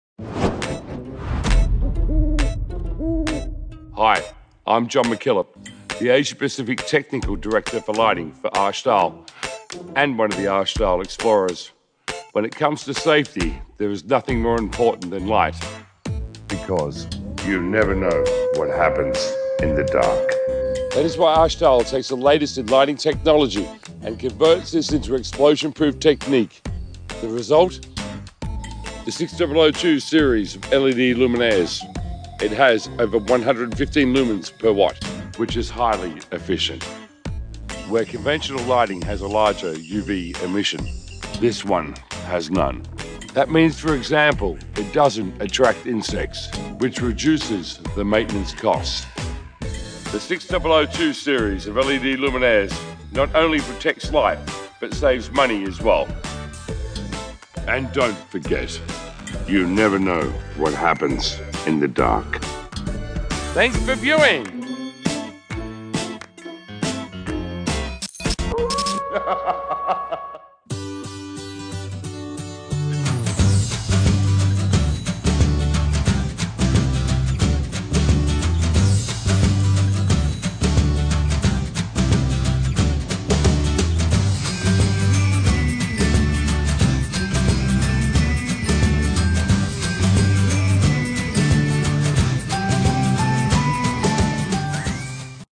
STAHL 6002強勢登台，防爆LED燈閃亮新星
適用區域 Zone 1, 2, 21, 22
防爆等級
Ex db eb op is IIC T4 Gb
Ex tb op is IIIC T100°C Db
防塵防水 IP 66
認證文件 產安技 (ISTI)2017第07-00059號
特別適用於煉油廠、石化業鑽油平台、其他危險區域室內外照明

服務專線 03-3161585